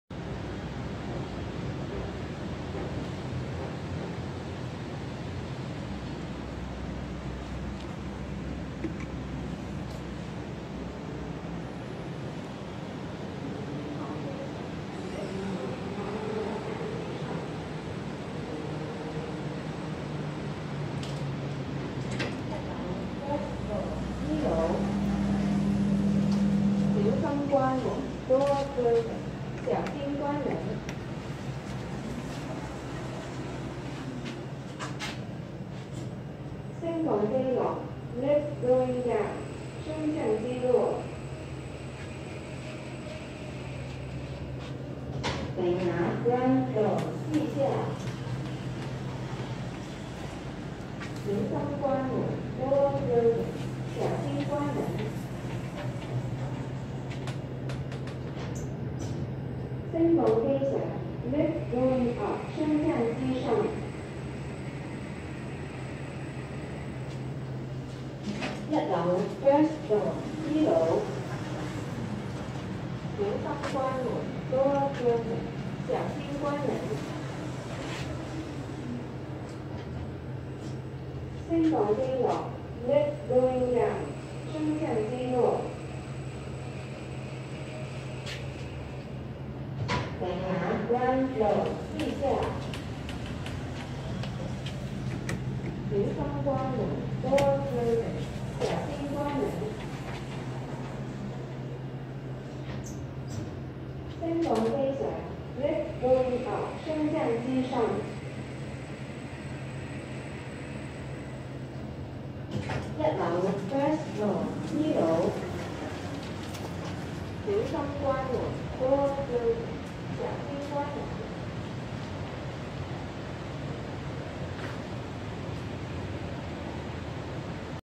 Schindler MRL Traction Elevator @Harbour Glory Footbridge, Fortress Hill
Address: 32 City Garden Rd, Fortress Hill, North Point, HK
Year of commission: 2019
Lift No.L32
Elevator maintenance: Schindler Elevator
Capacity: 1000KG - 13Persons
Via the floor: 2 (*G, 1)